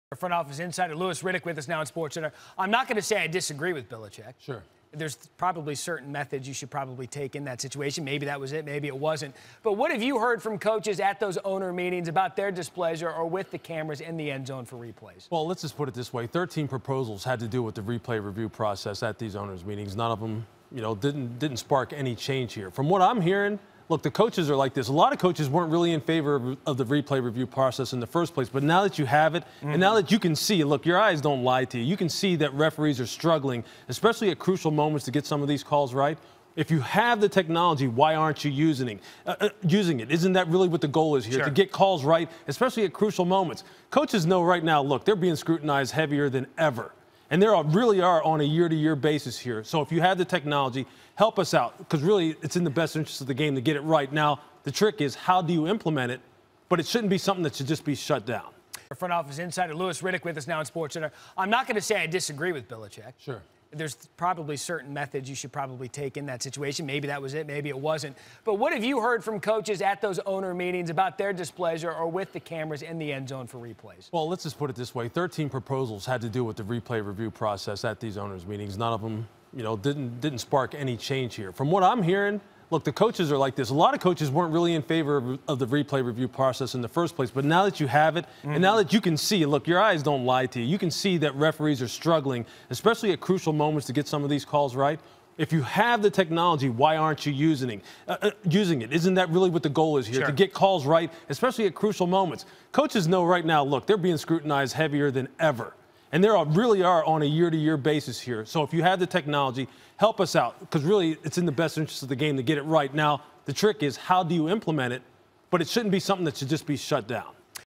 Belichick Upset With NFL Over End Zone Cameras
Louis Riddick reacts to Patriots coach Bill Belichick, who voiced his displeasure over the NFL not funding end zone cameras.